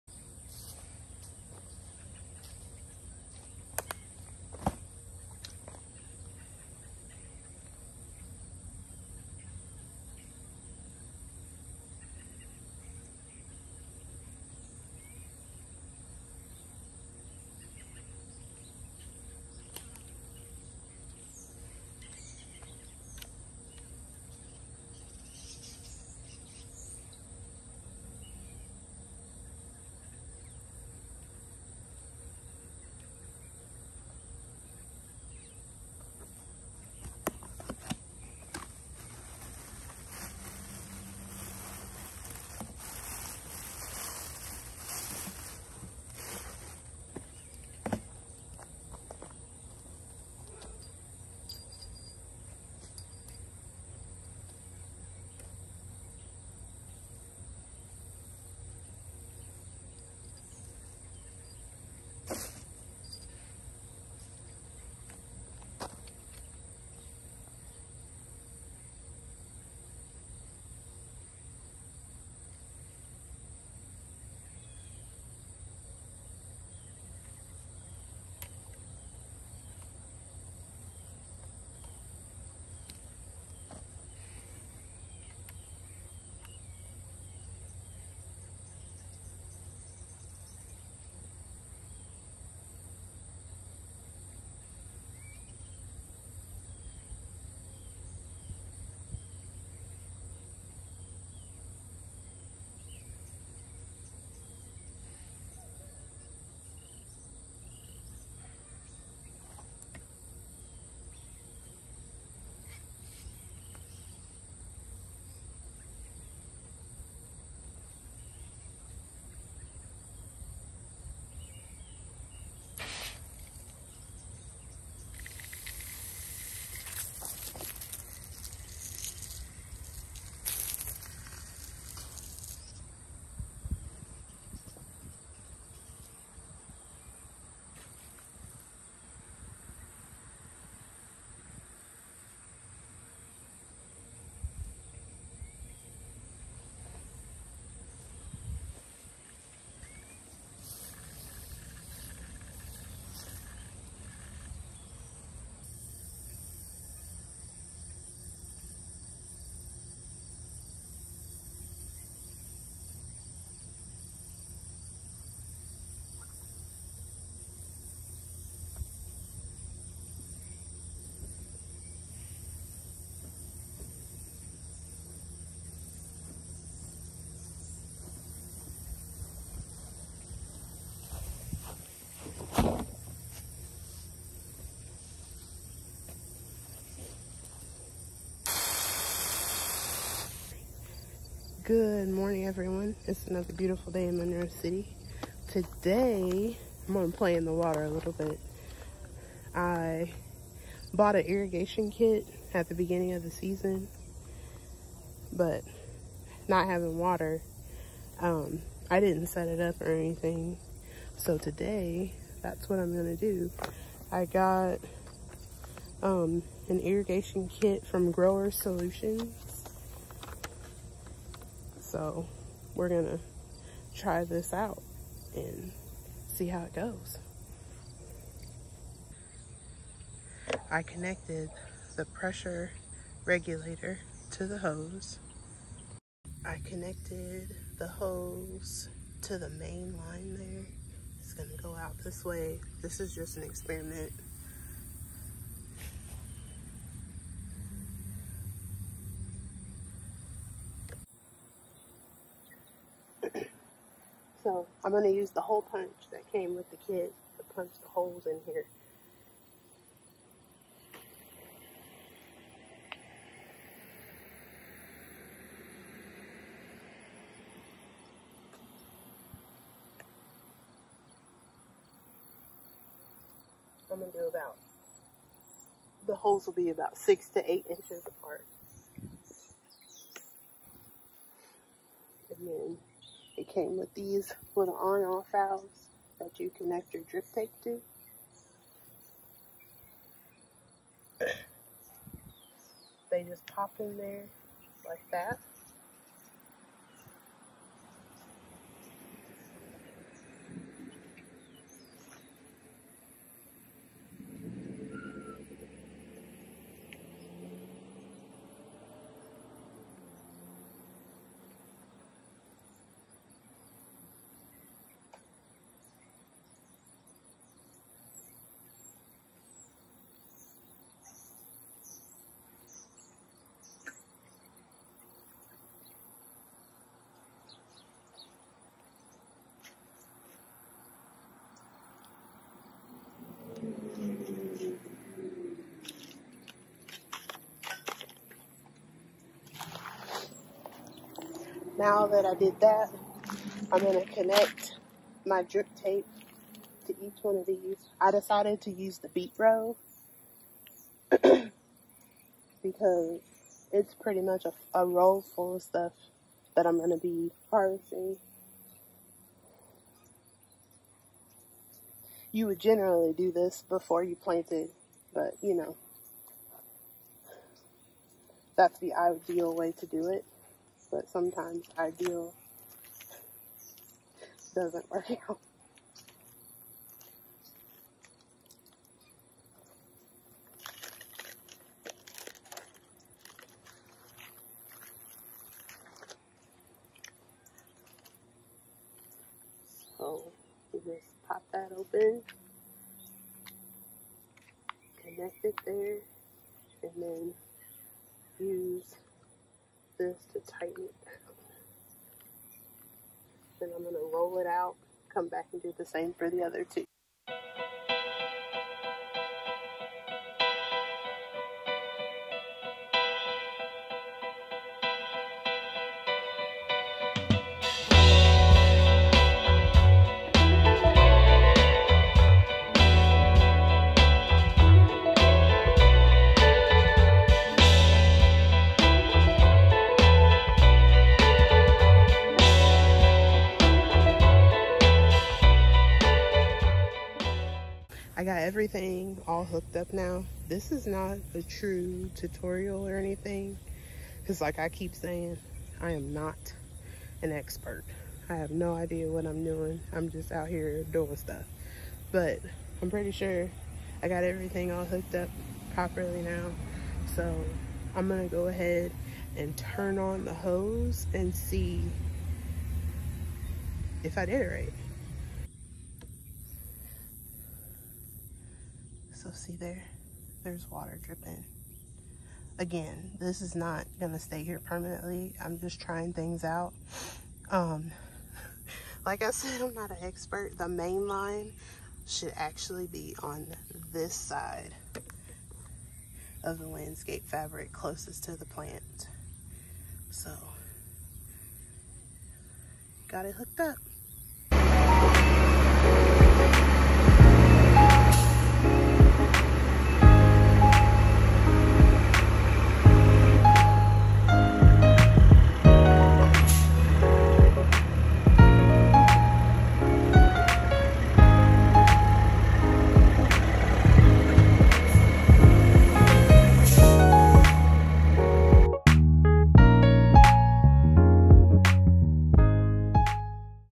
Irrigation Setup? (More of an Experiment) Vlog 9
In this video I experiment with drip irrigation and get a compost delivery. Like I keep saying I'm no expert; I'm just out here trying to grow food, build a business, and enrich my community. Thanks for going along with me on this journey!